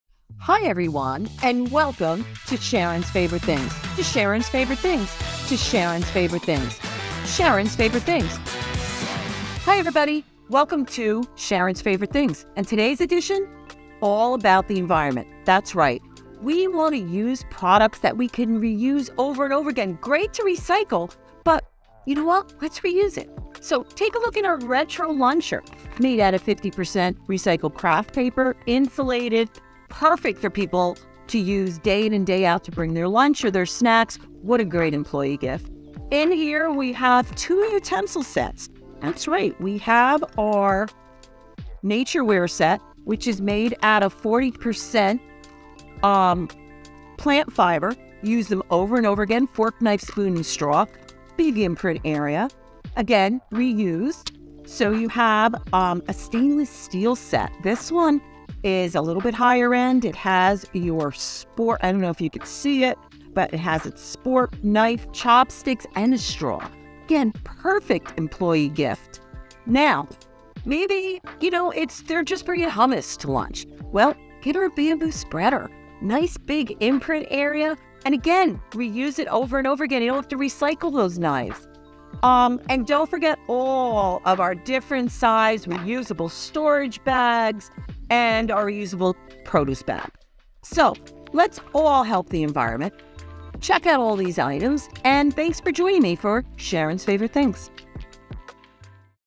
Reduce & Reuse | Sharon's Favorite Things
In this episode of Sharon's Favorite Things, she shares environmentally friendly products! Find them online here: